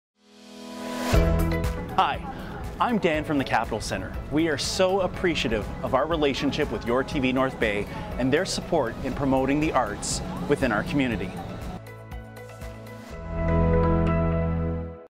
What role does YourTV play in our community?
Dan Misturada is the Executive Director @capitolcentre in North Bay. He appreciates the role @yourtvnorthbay plays in promoting the arts within our community. arts trulylocal @yourtvontario @cogecohelps @cogeco